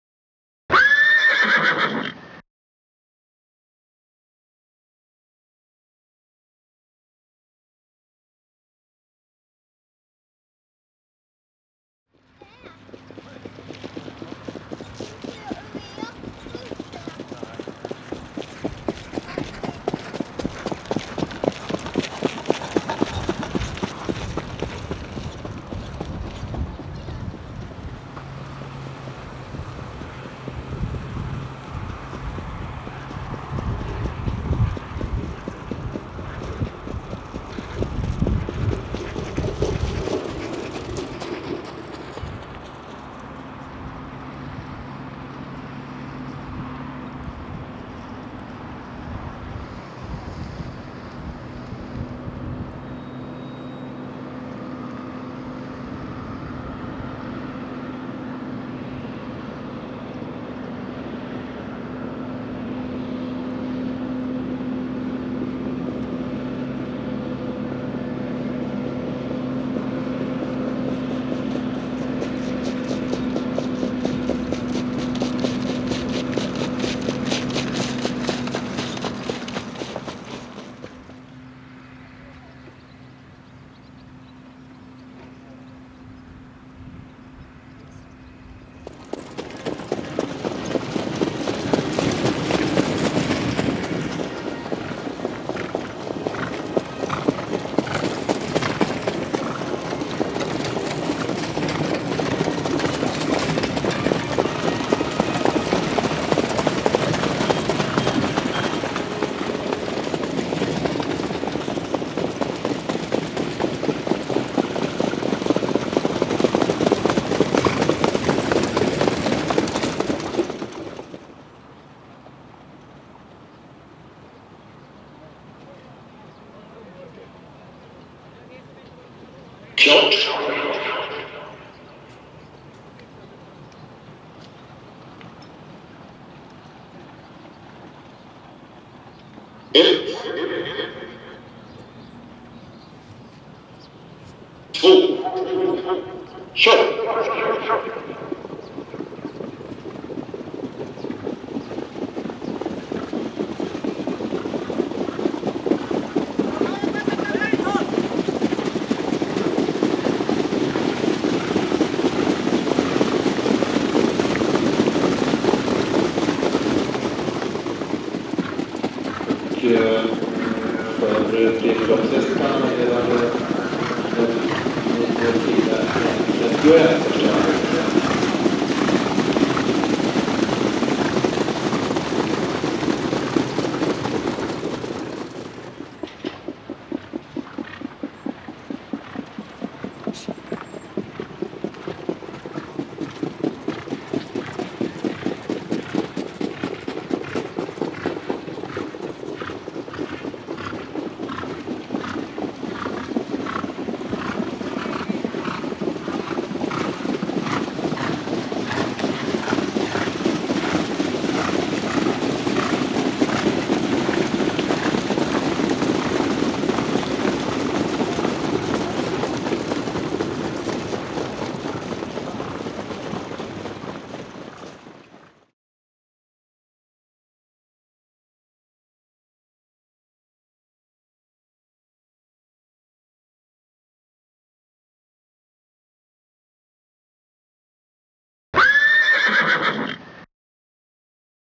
Checkpoint går premielopp på Örebro 18/8 2011
Checkpoint efter Donato Hanover går premielopp på Örebro travet den 18 augusti 2011, i sulkyn sitter Per Fromell.